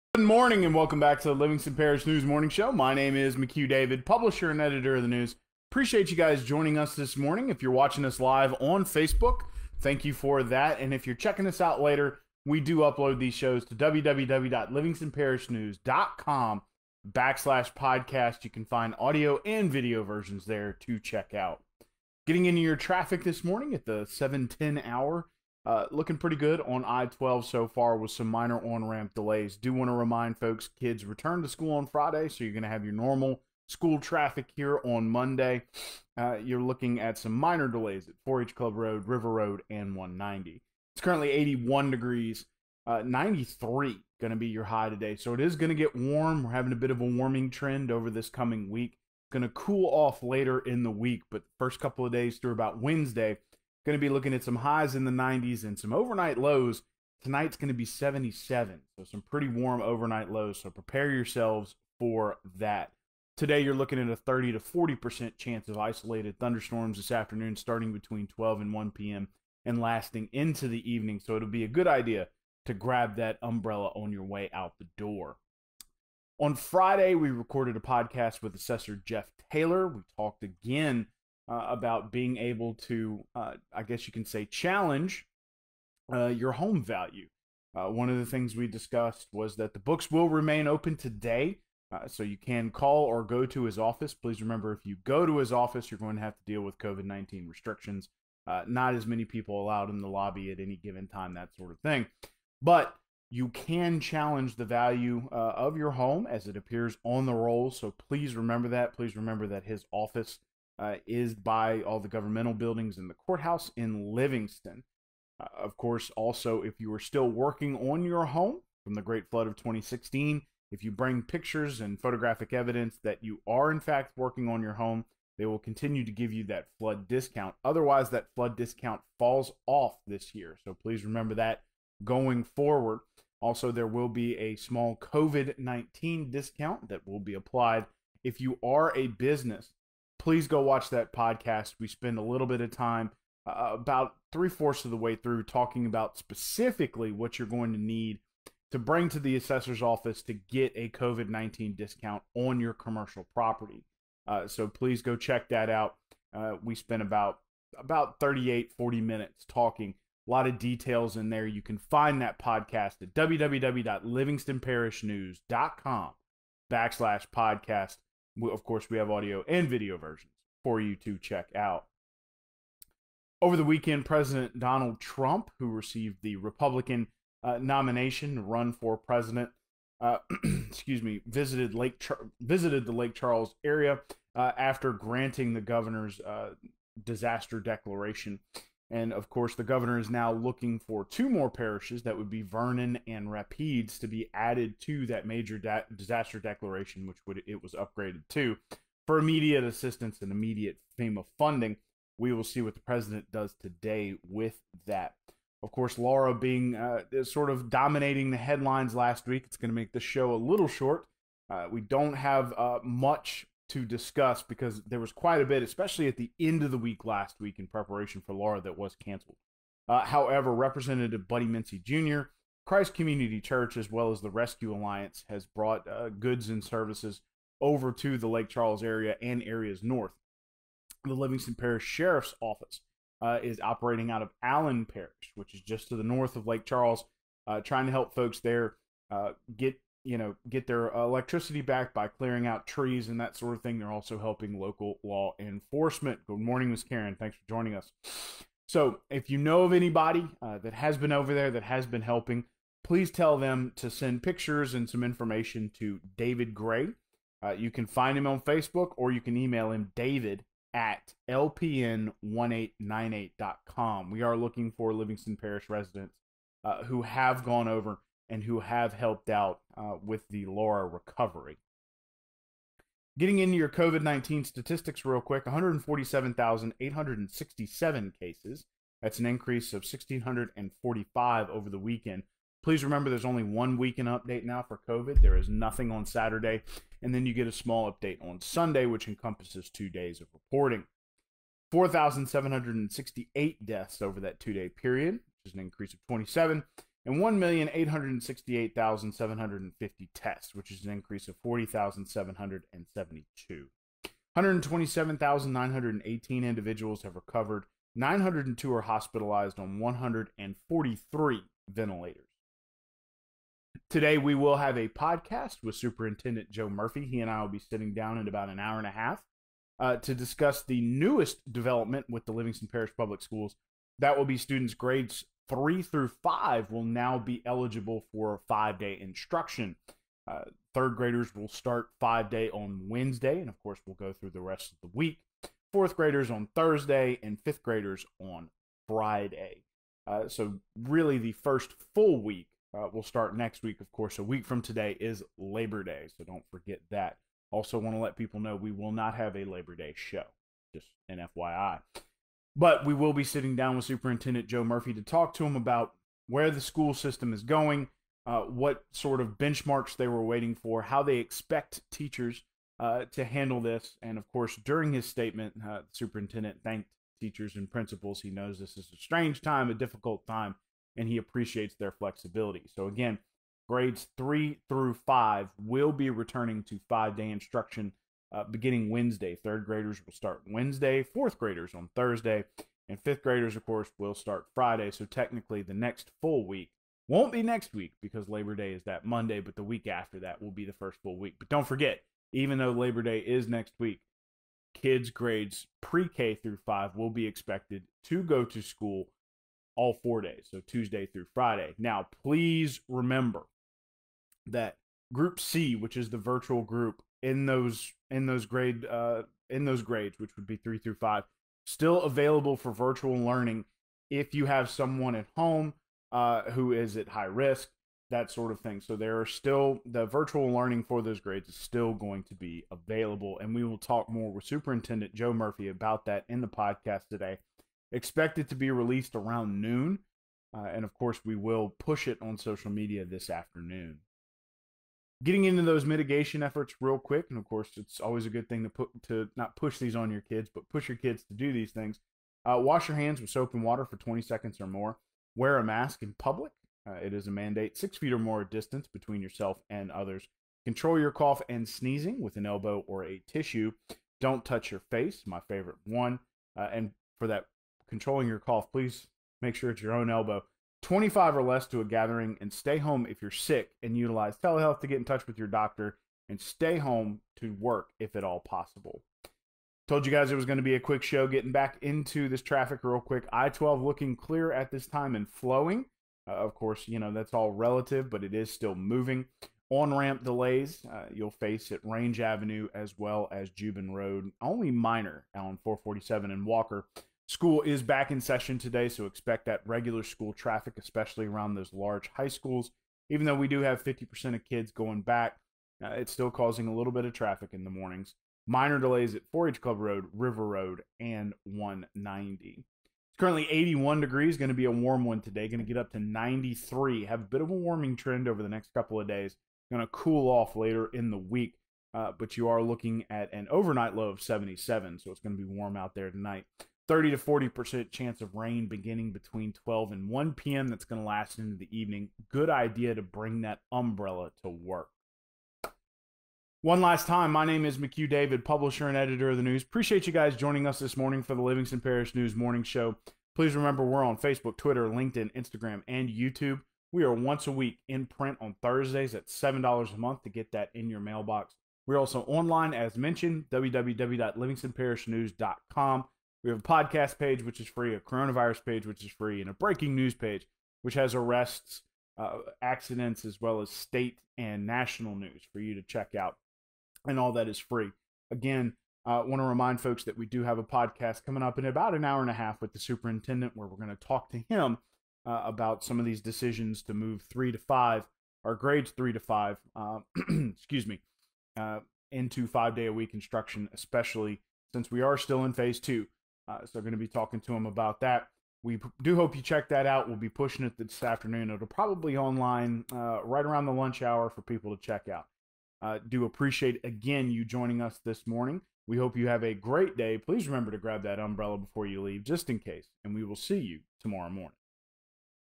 PODCAST | The Livingston Parish News Morning Show - August 31, 2020!
Publisher and editor McHugh David delivers the Livingston Parish traffic, weather, headlines, and more for the Livingston Parish News Morning Show on August 31, 2020!